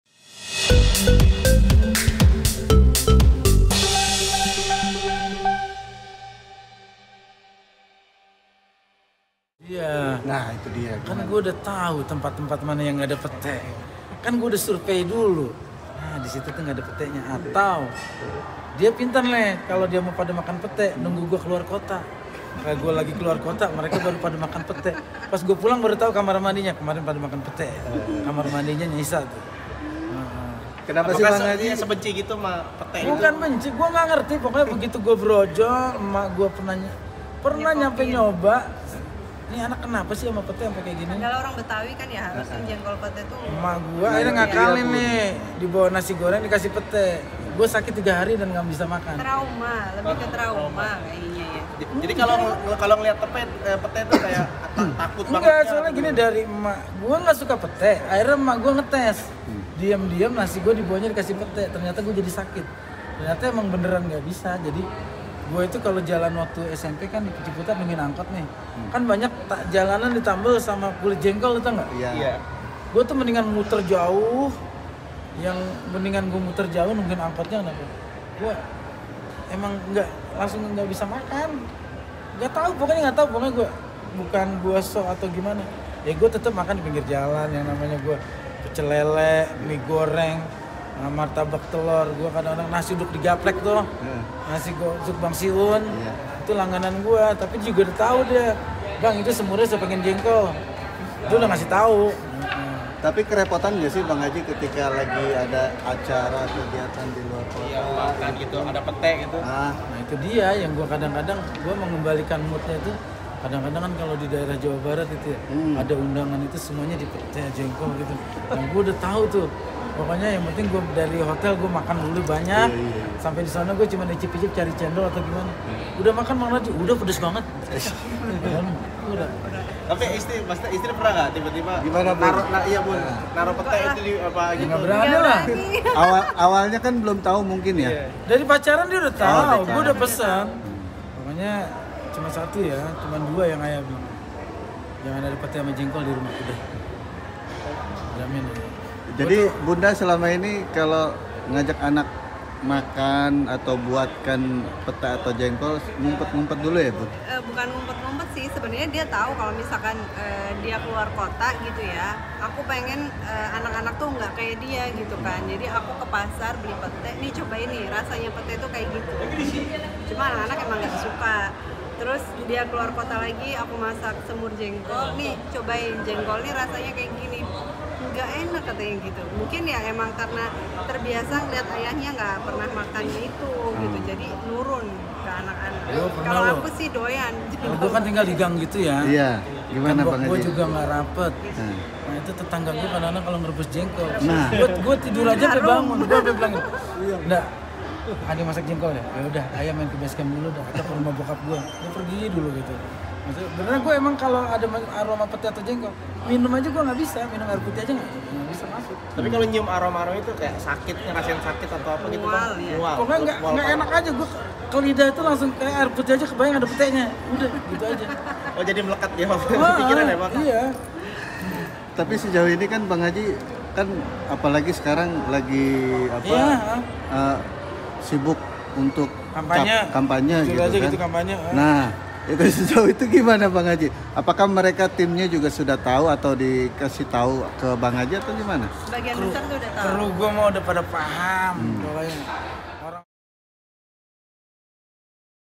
DARI KECIL NARJI TAK MENYUKAI PETE
Narji memang sangat tidak menyukai pete padahal sebagai orang betawi pete dan jengkol itu jadi makanan favorit. Narji pun bercerita kalau dirinya pun bingung kenapa dia tak suka pete.